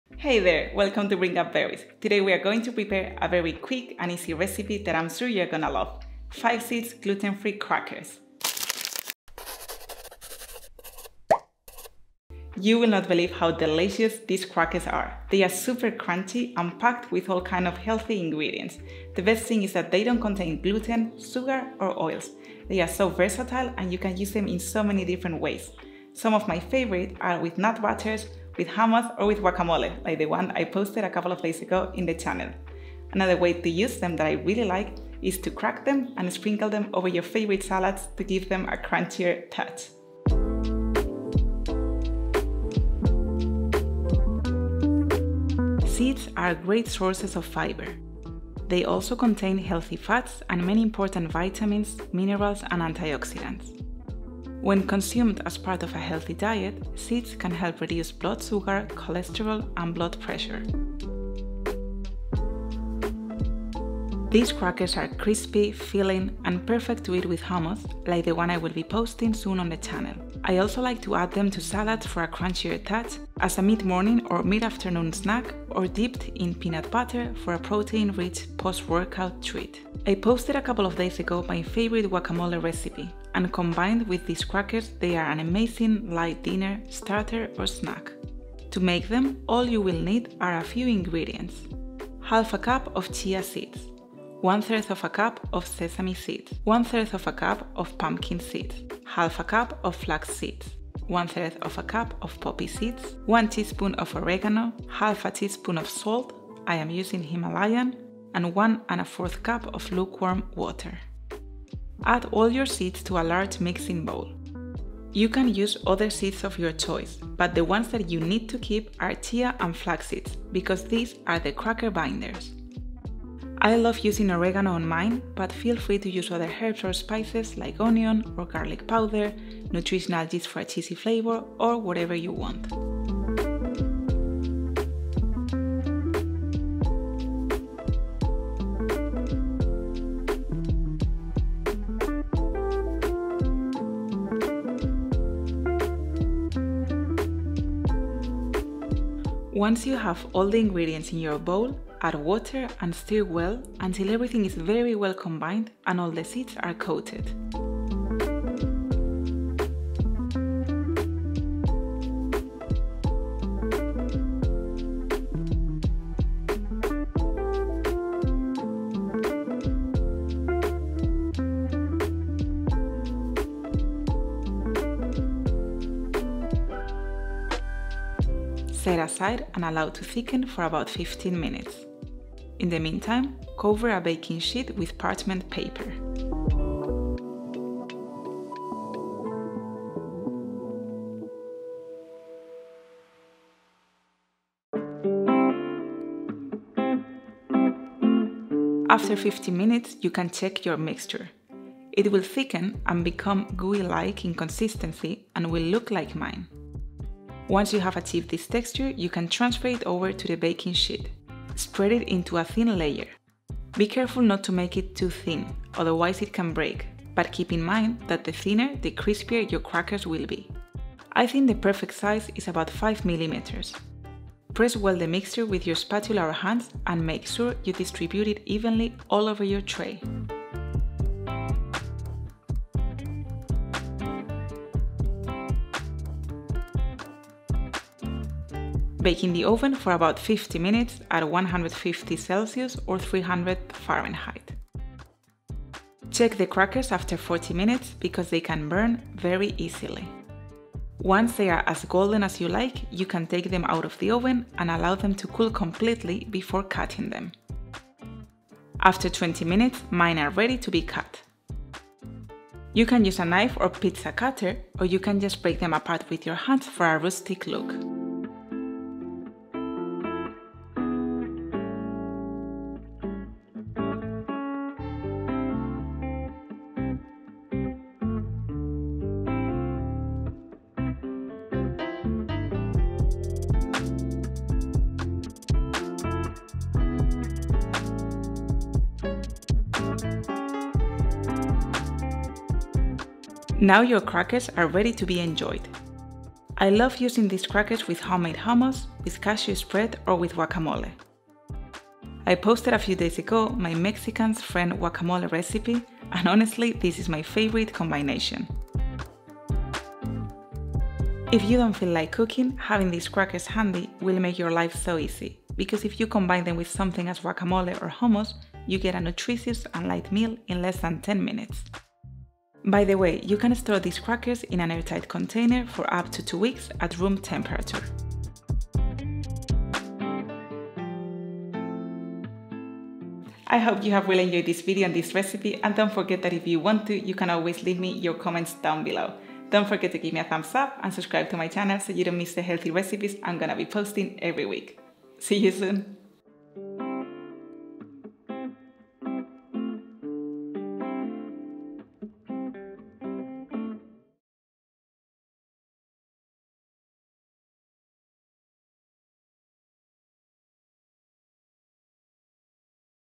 SUPER CRISPY GLUTEN-FREE CRACKERS. NO FLOUR, NO OILS. JUST SEEDS. Fast and easy recipe.
Welcome to Bring up Berries, today we are going to prepare a delicious recipe, very quick and easy. Gluten and flour-free seed crackers.
You won't believe how delicious these crackers are. they are super crunchy and packed with nutrients and healthy ingredients. They are gluten, oil, and sugar-free and can be used in a thousand different ways. My favorites are spreads, hummus, nut butter, or guacamole, or tossed on top of salads for crunch.

INGREDIENTS:
80 g- 1/2 cup of chia seeds
50 gr- 1/3 cup of sesame seeds
50 gr- 1/3 cup of poppy seeds
80 gr - 1/2 cup of flax seeds
40 g- 1/3 cup of pumpkin seeds
1 tsp oregano (optional)
1/2 tsp salt (optional)
1 1/4 cups lukewarm water

My measuring cup is 250 ml

CHAPTERS
00:00 intro
01:37 Ingredients
01:56 Recipe
05:38 See you soon!